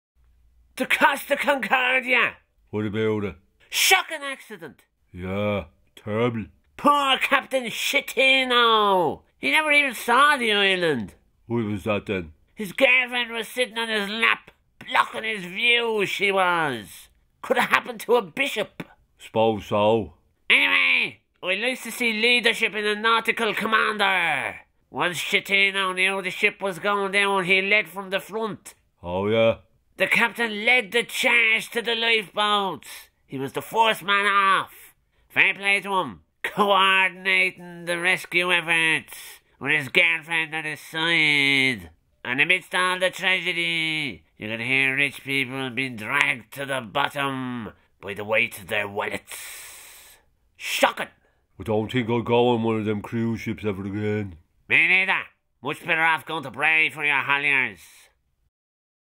Costa Concordia!
Dominic frets about the shocking Costa Concordia accident... Shocking!